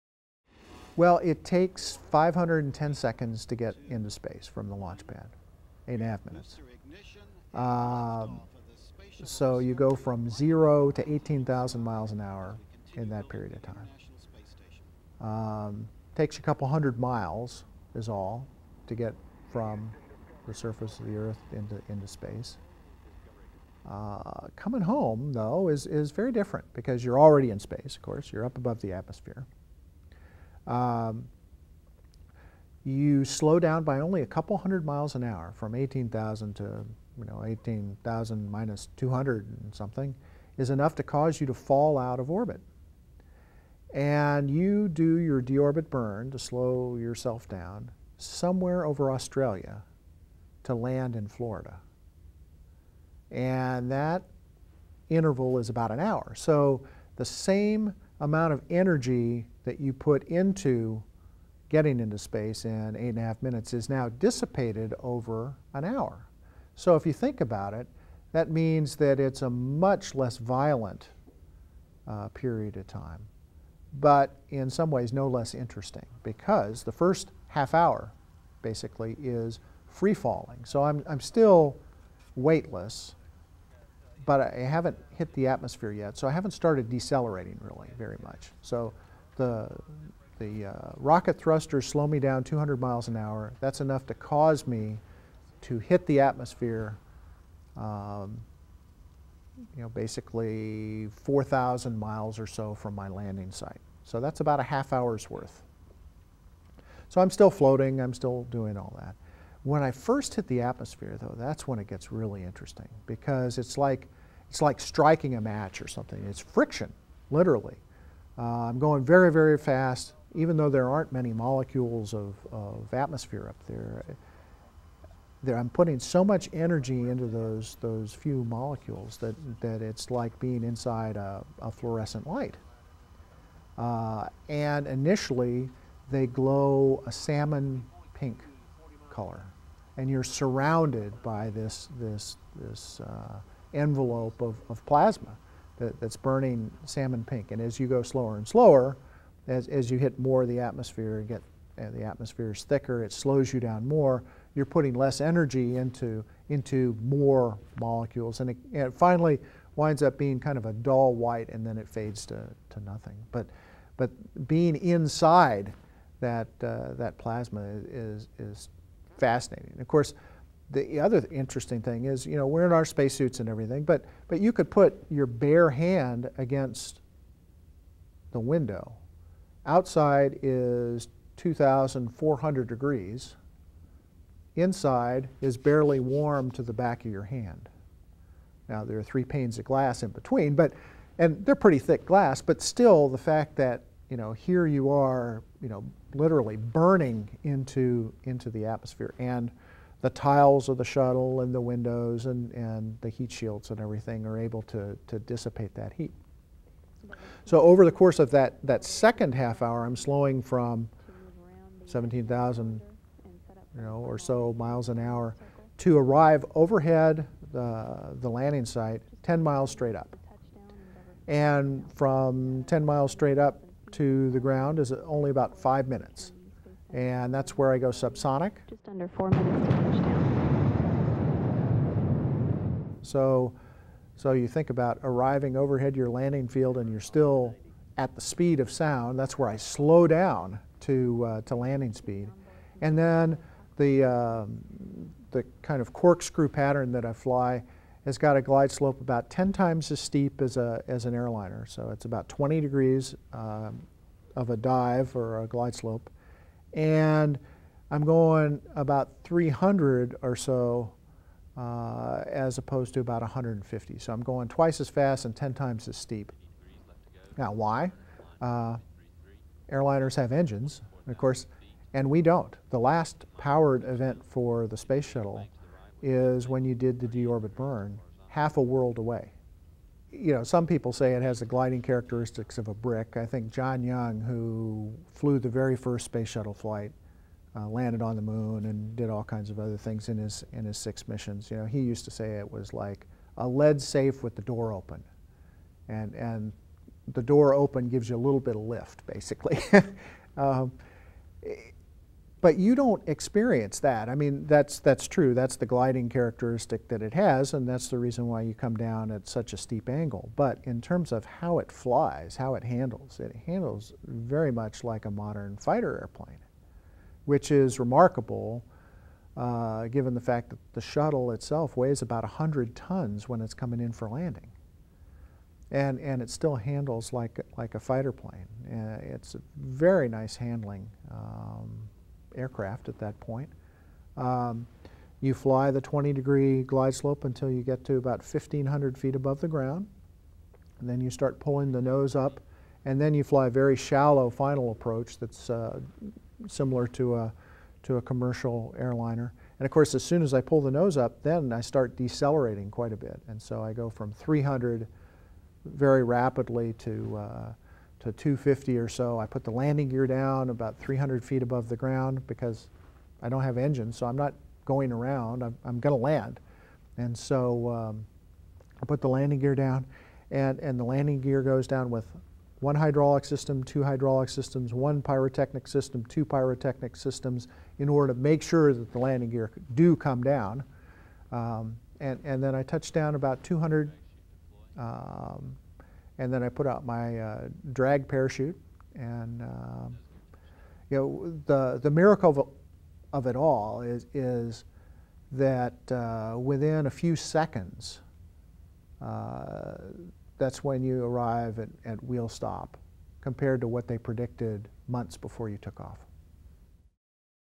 What is the Space Shuttle landing like?
Former NASA astronaut William F. “Bill” Readdy answers questions during Flight School: Blast Off!!